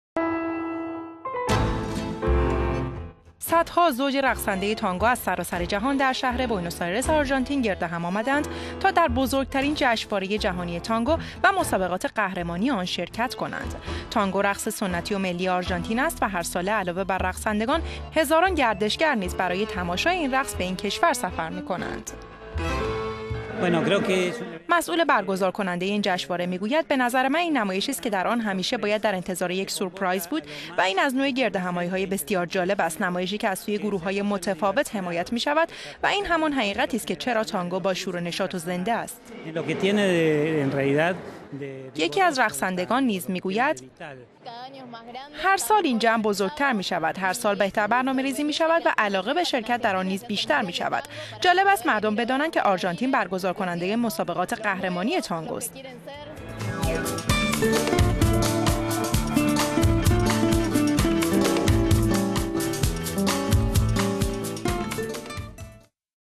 جشنواره جهانی تانگو در آرژانتین
صد‌ها زوج رقصنده تانگو از سراسر جهان در شهر بوئنوس آیرس آرژانتین گرد هم آمده‌اند تا در بزرگترین جشنواره جهانی تانگو و مسابقات قهرمانی آن شرکت کنند.
تانگو، رقص سنتی و ملی آرژانتین است و هرساله علاوه بر رقصندگان هزاران گردشگر نیز برای تماشای این رقص وبه این کشور سفر می‌کنند.

گوستاو موسی، مسئول برگزار کننده این جشنواره می‌گوید: «به نظر من این نمایشی است که در آن همیشه باید در انتظار یک سورپرایز بود. نمایشی است که از سوی گروههای متفاوت حمایت می‌شود و این حقیقت است که تانگو همیشه باشور و نشاط و زنده است.»

رقص تانگو برای نخستین بار در قرن ۱۹ میلادی در حومه بوئنوس آیرس و در یکی از محله‌های پایین دست این شهر به وجود آمد و سپس به آرامی به تئاتر‌ها راه یافت، و از محله‌های فقیر نشین و از میان مردم معمولی به کلاس‌های رقص سطح بالا راه پیدا کرد.
این جشنواره از چهارهم ماه اوت آغاز شده و تا بیست هشتم این ماه ادامه خواهد یافت.